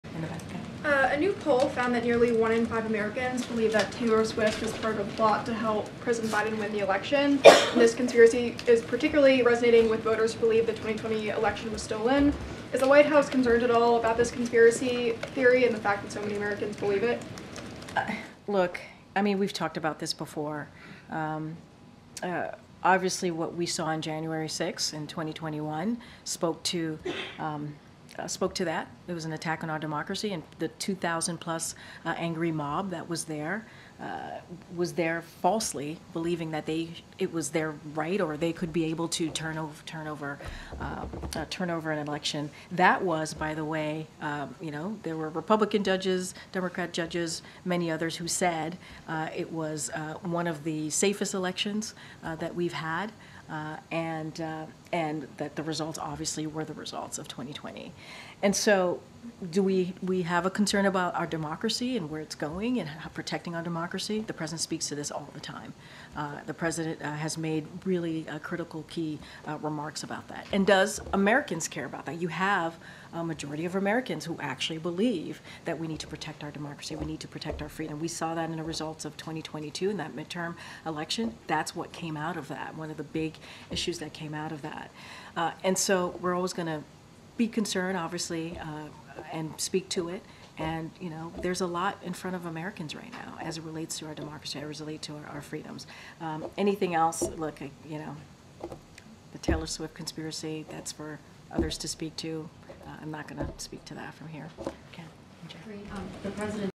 WH Pressed On Poll Showing 1 in 5 Americans Think Taylor Swift Is Plotting To Help Biden Win In 2024
During a White House press briefing on Wednesday, Karine Jean-Pierre was asked about a poll that shows some Americans believe Taylor Swift is part of a plot to help Biden win reelection.